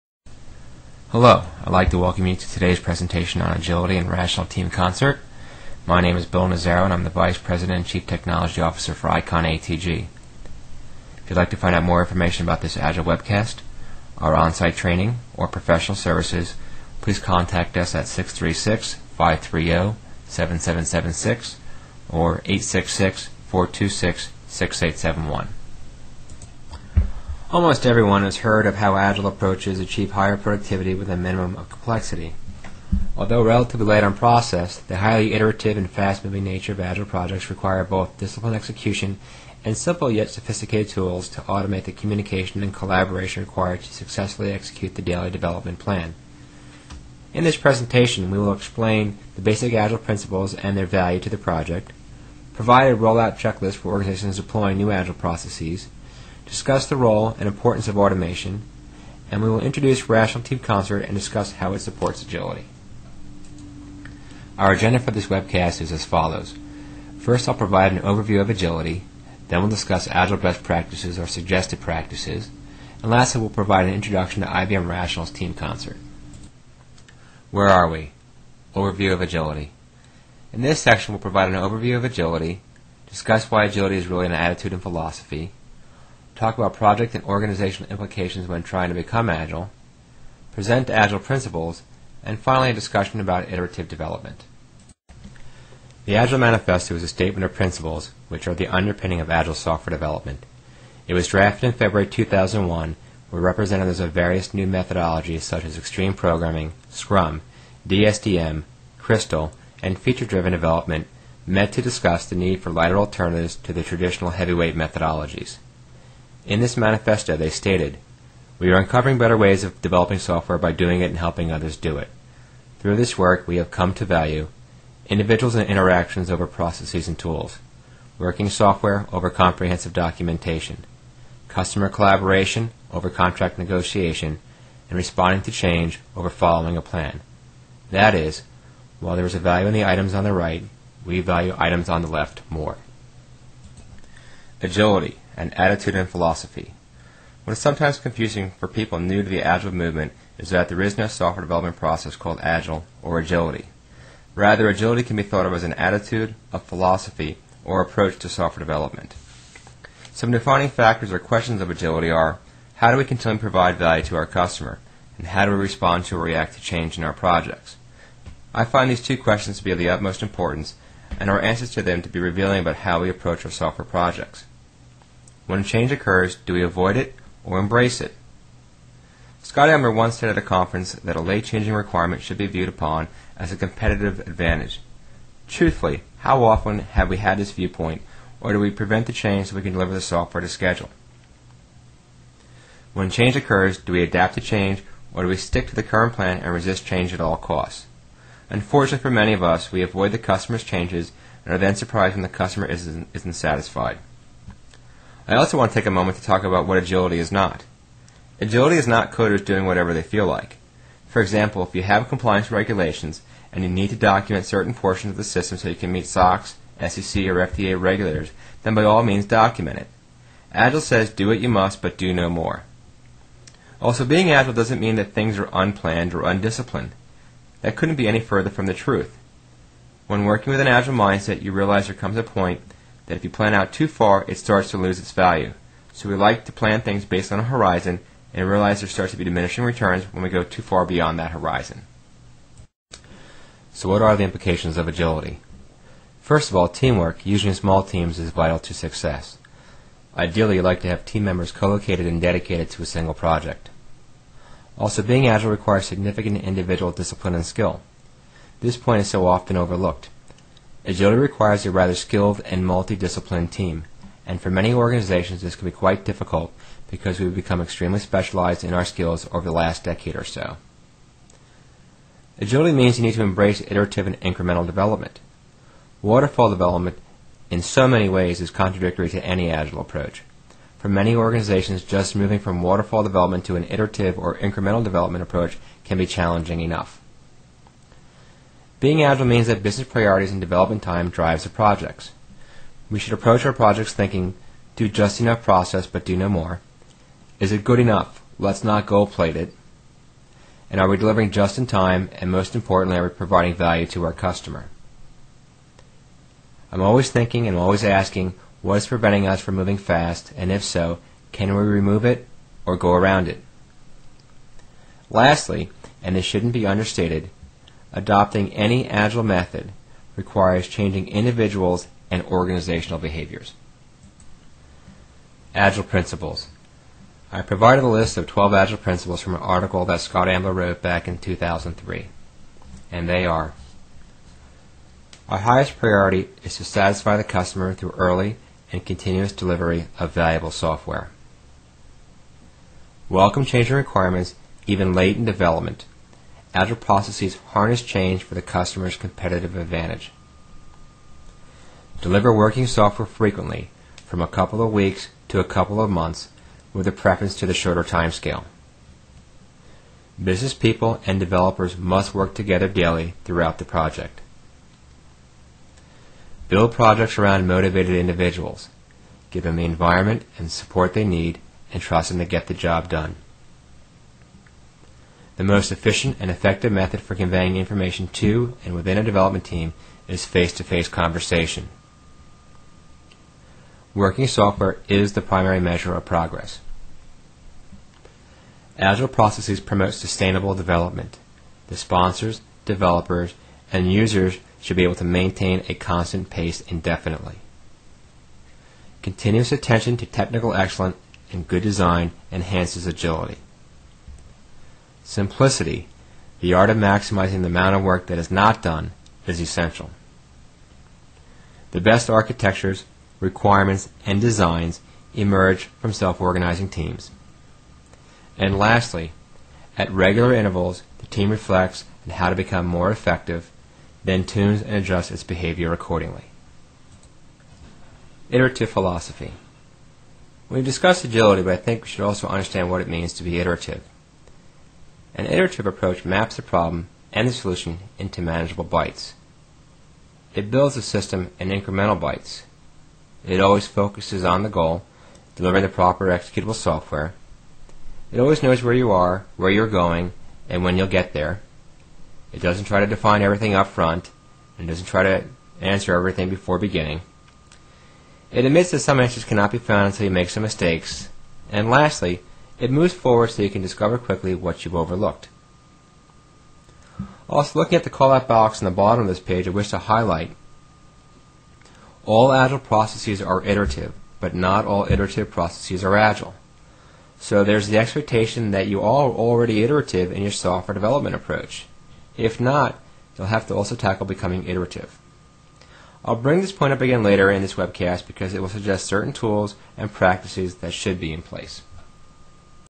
Agility and Rational Team Concert | Part 1
This 3 part series provides a basic overview of agile software development, the most common best practices, and Rational Team Concert (which provides build management, software configuration management, work item management, and reporting capabilities).